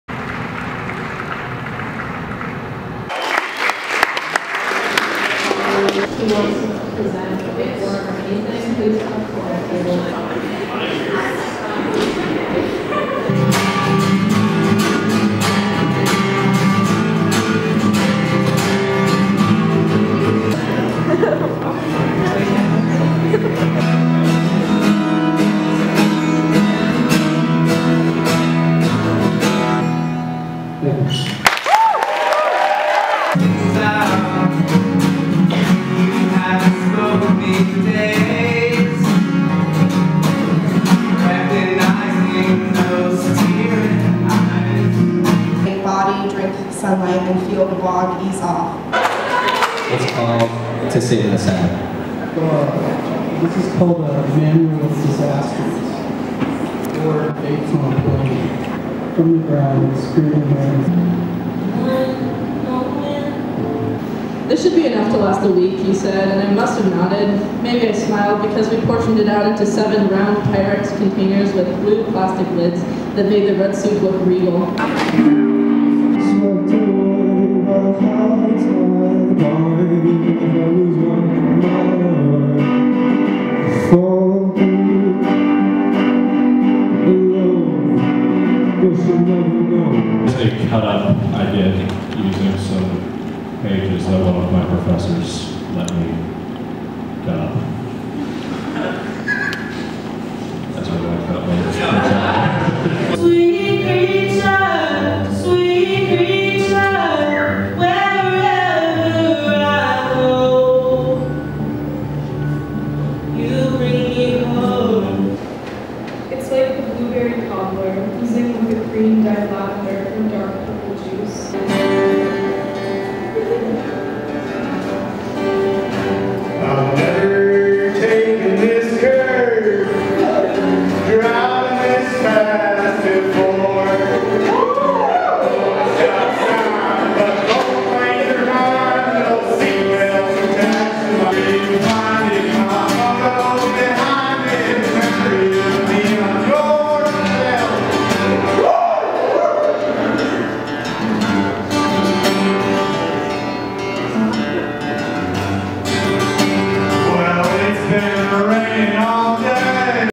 KUPS Oppenheimer Open Mic 2019
This video is for KUPS 90.1 FM's open mic, hosted by Oppenheimer Cafe, on November 12th, 2019.